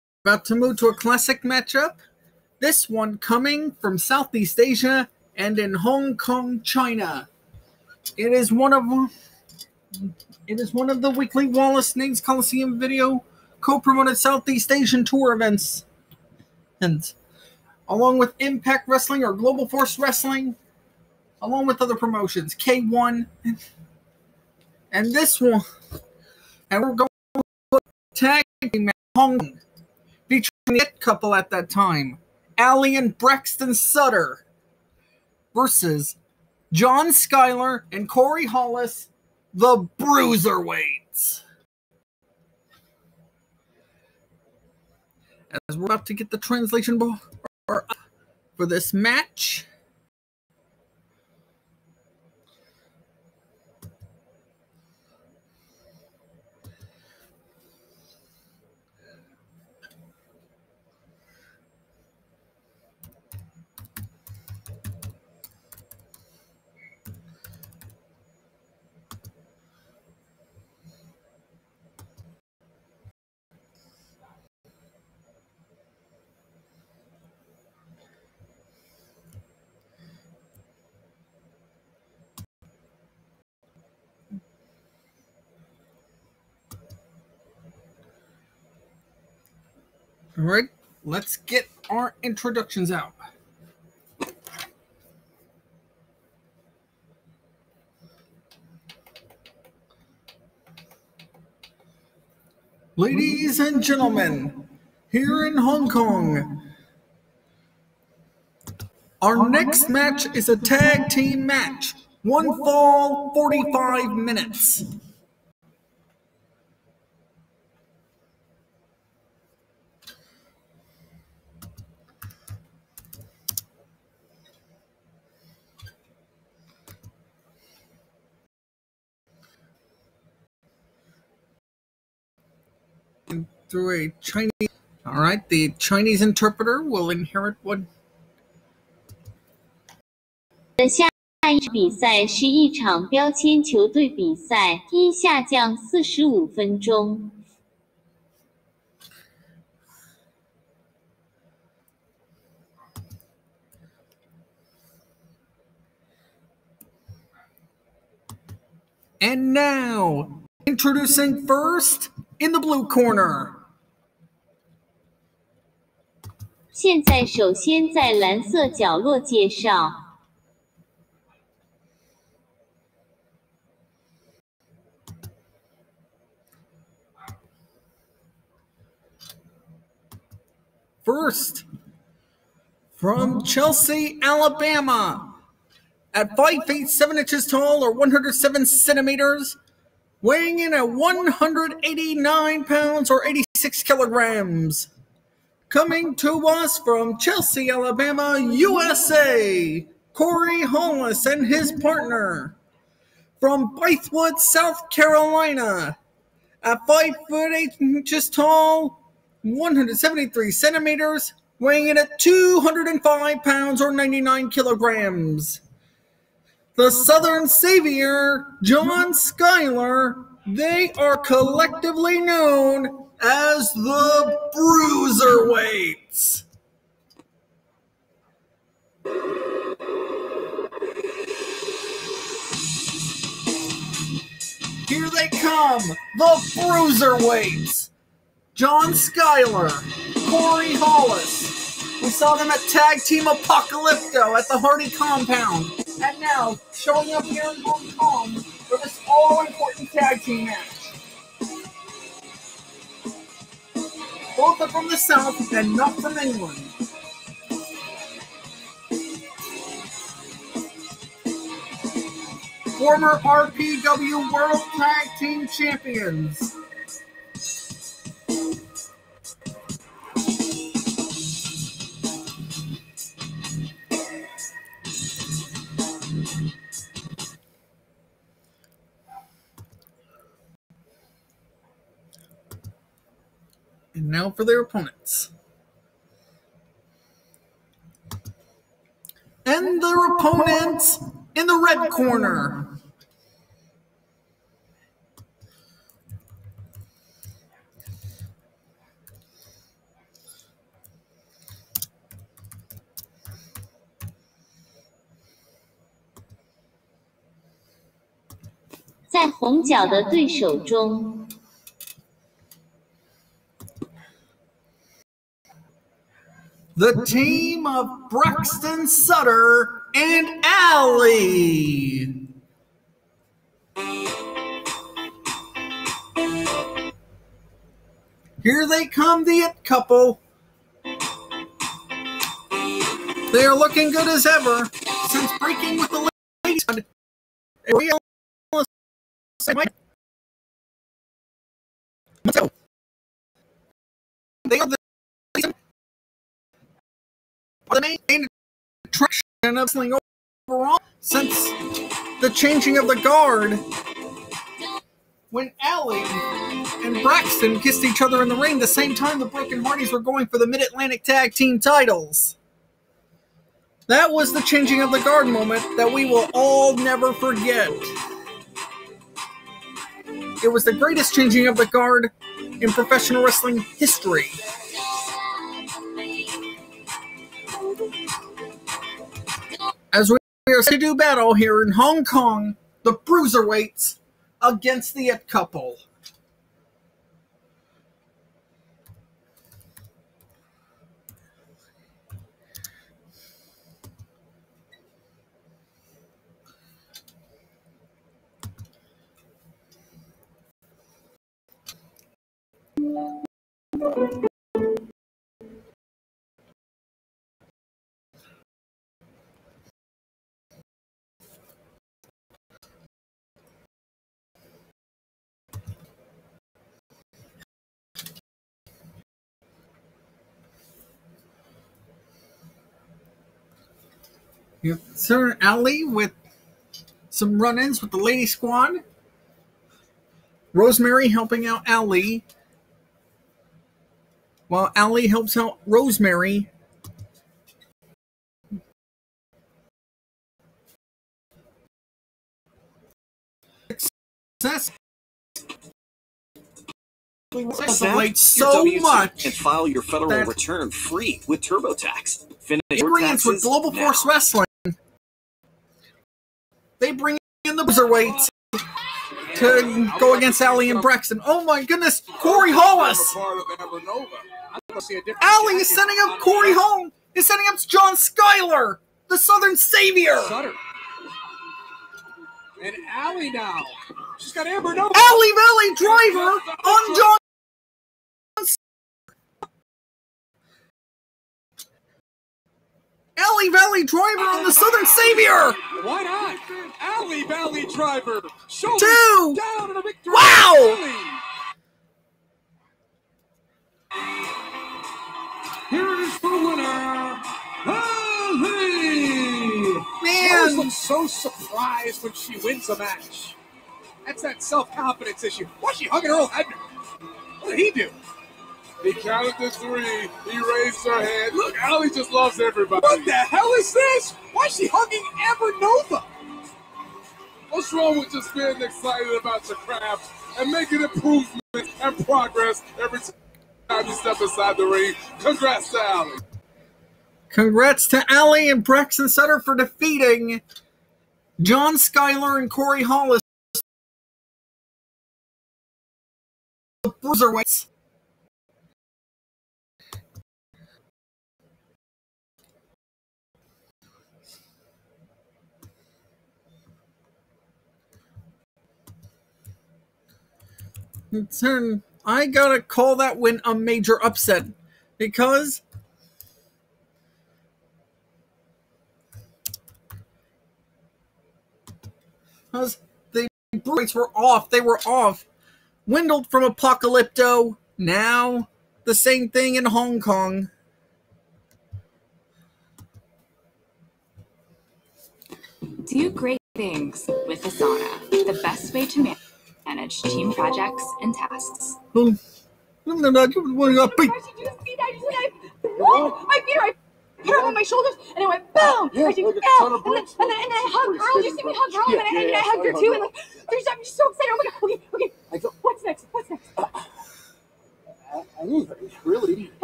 Allie and Braxton Sutter vs The Bruiserweights
this classic match takes us to Hong Kong in June 2017, as the It Couple of Allie and Braxton Sutter take on the Southern Savior John Skyler and Corey Hollis collectively known as the Bruiserweights